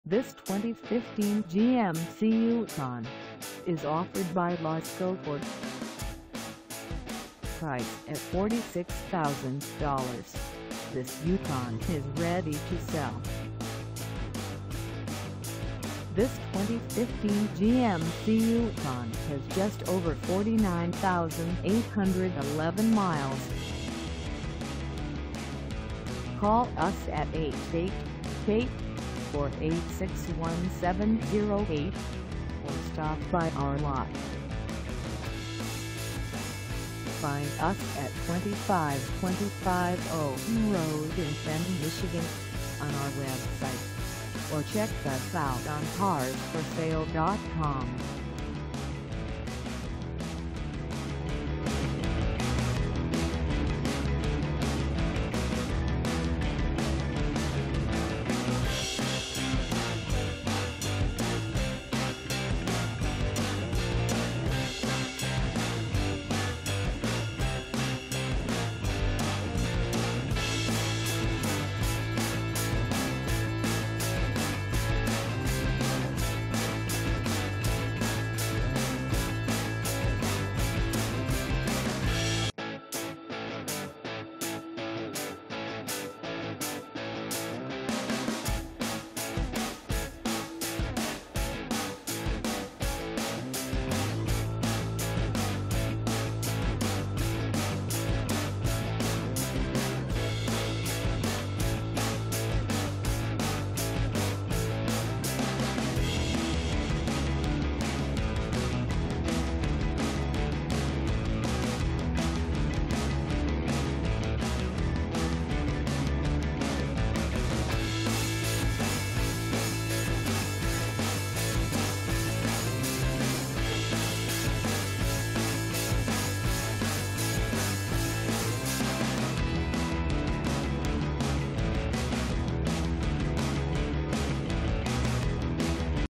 2015 GMC Yukon Denali 4x4 4dr SUV for sale in FENTON, MI 484
This 2015 GMC Yukon Denali 4x4 4dr SUV is for sale in FENTON, MI 48430 at LASCO FORD.

Engine: 6.2L V8
Transmission: Automatic
Color: Black
Mileage: 49811
Contact LASCO FORD 2525 OWEN RD FENTON, MI 48430 to test drive this 2015 GMC Yukon Denali 4x4 4dr SUV today.